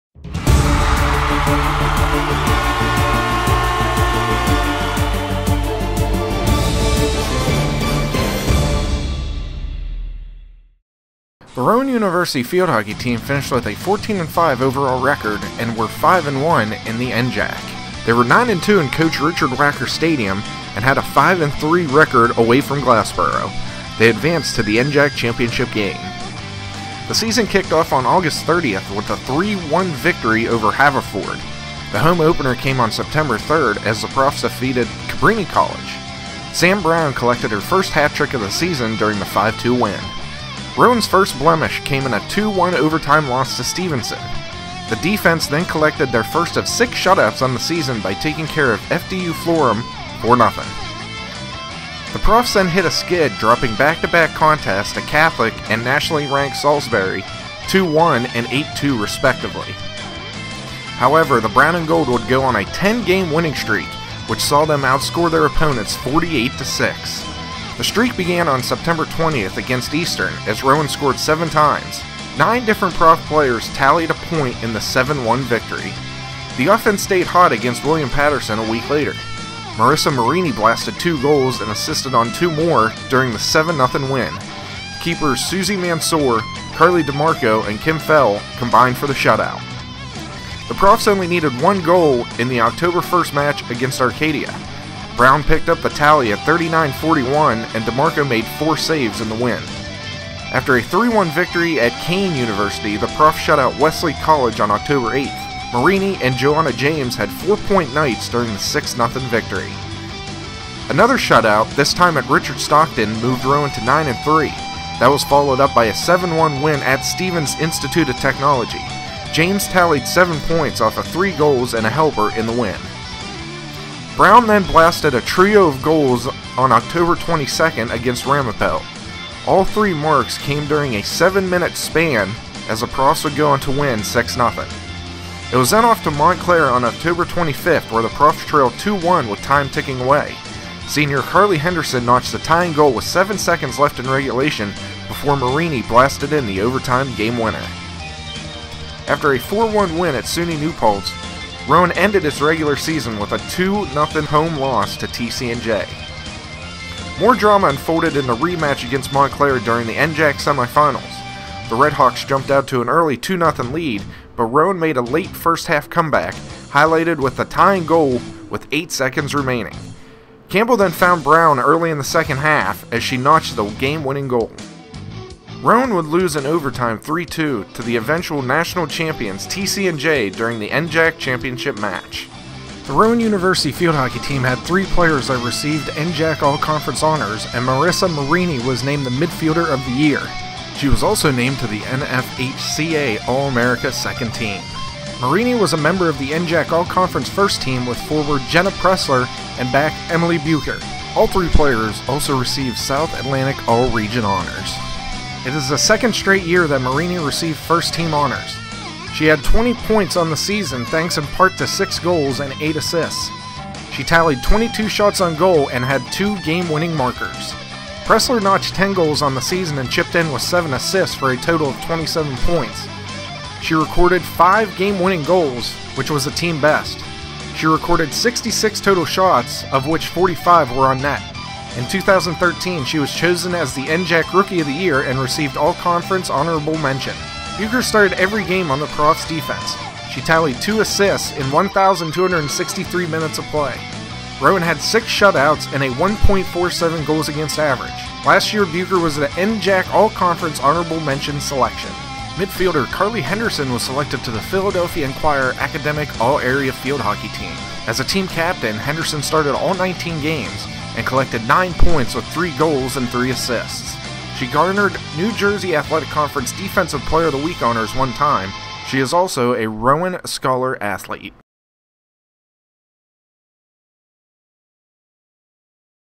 2014 Rowan Field Hockey - Season Review
The Profs finished 14-5 overall, advancing to the NJAC finals.